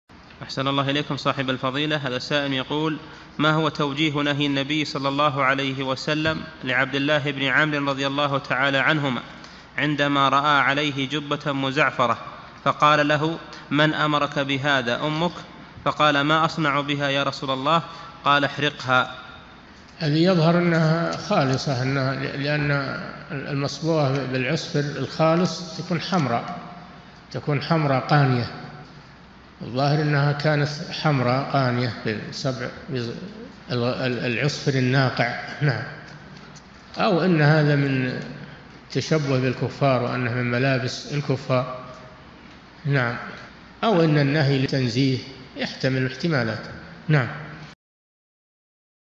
[771 -954] ما هو توجيه نهي النبي ﷺ لعبد الله بن عمرو أن يلبس جبة مزعفرة؟ - الشيخ صالح الفوزان
-[771 /954] السؤال: ما هو توجيه نهي النبي ﷺ لعبد الله بن عمرو رضي الله عنهما عندما رأى عليه جبة مزعفرة، فقال له: من أمرك بهذا أمك؟ فقال: ما أصنع بها يا رسول الله؟ قال: احرقها»؟
الجواب: الذي يظهر أنها خالصة، لأن المصبوغة بالعصفر الخالص تكون حمراء قانية، الظَّاهر أنها كانت حمراء قانية العصفر الناقع، أو أن هذا من التشبه بالكفار، وأنه من ملابس الكفار، أو أن النهي للتنزيه يحتمل احتمالات.
للاشتراك في قناة الشيخ صالح الفوزان
سؤال وجواب من شرح منظومة الآداب ( 945 ) مقطع

للاستفسار والإقتراحات والدعم
رئيس الفريق
لمشروع كبار العلماء